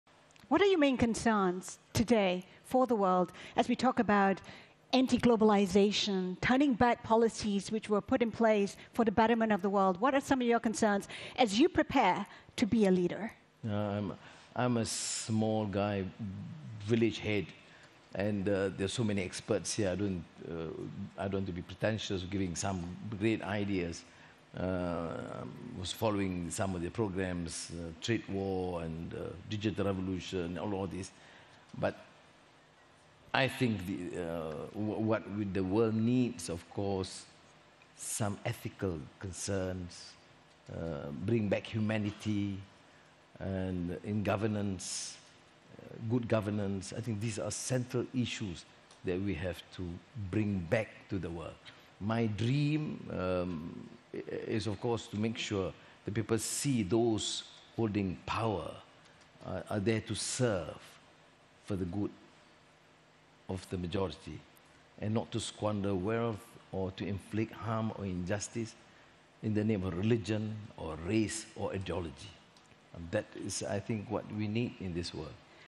Anwar Ibrahim : Anti globalisation sentiments can be allayed by serving the majority of the people.
Source: Bloomberg